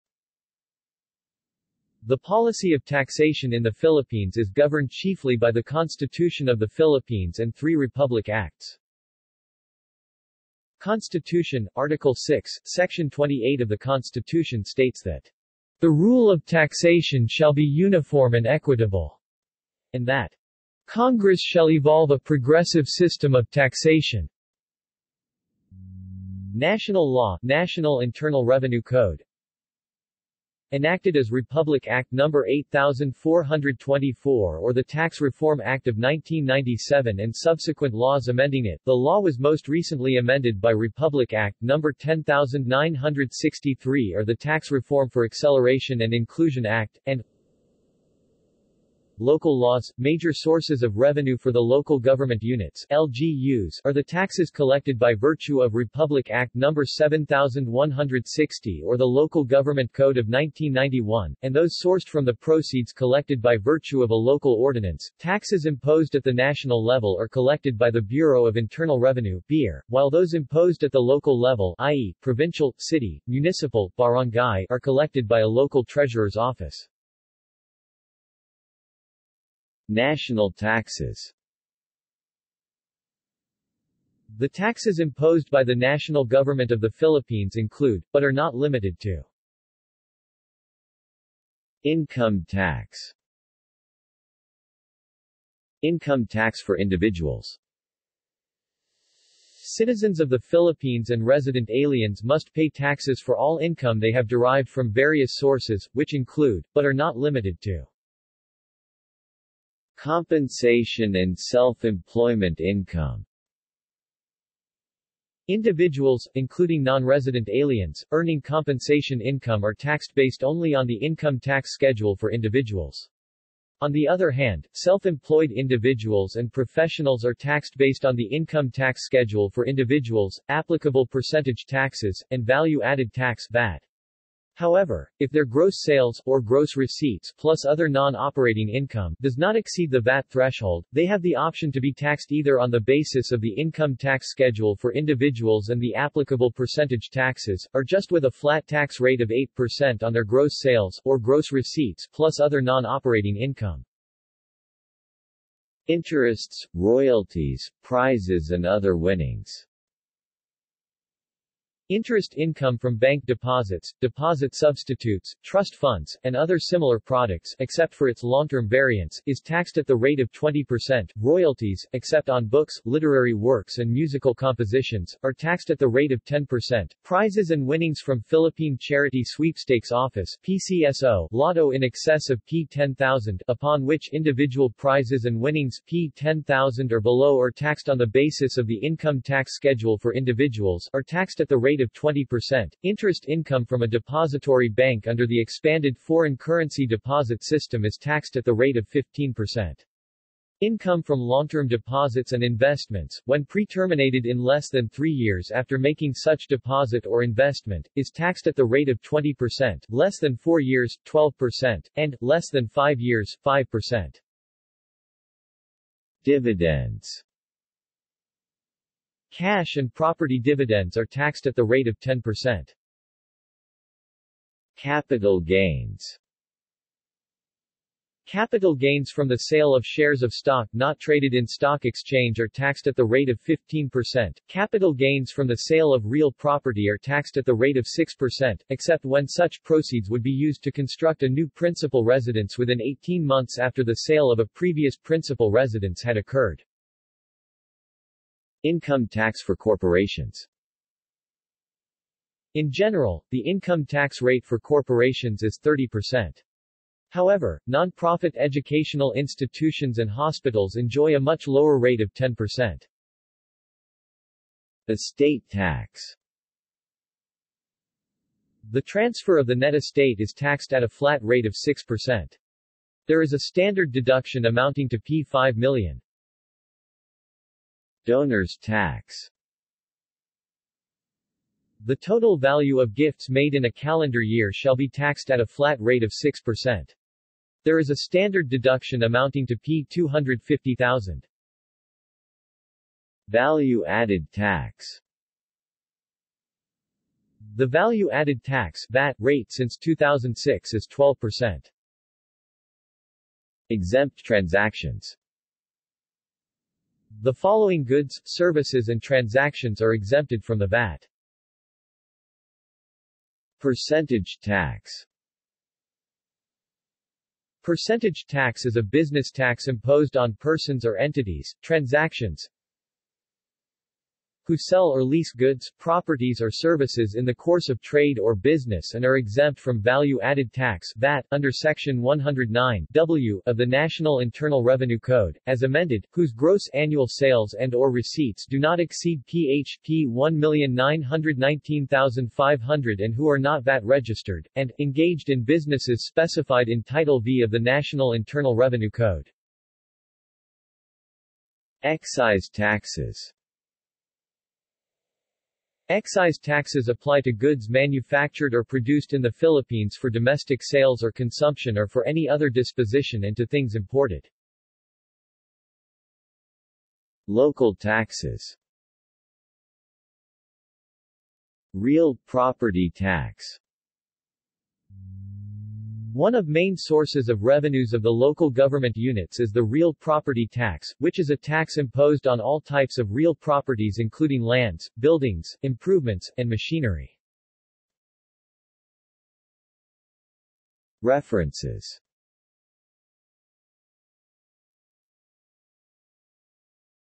Taxation in the Philippines | Wikipedia audio article
This is an audio version of the Wikipedia Article:
Taxation in the Philippines

SUMMARY
The policy of taxation in the Philippines is governed chiefly by the Constitution of the Philippines and three Republic Acts.

Constitution: Article VI, Section 28 of the Constitution states that "the rule of taxation shall be uniform and equitable" and that "Congress shall evolve a progressive system of taxation."
national law: National Internal Revenue Code—enacted as Republic Act No. 8424 or the Tax Reform Act of 1997 and subsequent laws amending it; the law was most recently amended by Republic Act No. 10963 or the Tax Reform for Acceleration and Inclusion Act; and,
local laws: major sources of revenue for the local government units (LGUs) are the taxes collected by virtue of Republic Act No. 7160 or the Local Government Code of 1991, and those sourced from the proceeds collected by virtue of a local ordinance.Taxes imposed at the national level are collected by the Bureau of Internal Revenue (BIR), while those imposed at the local level (i.e., provincial, city, municipal, barangay) are collected by a local treasurer's office.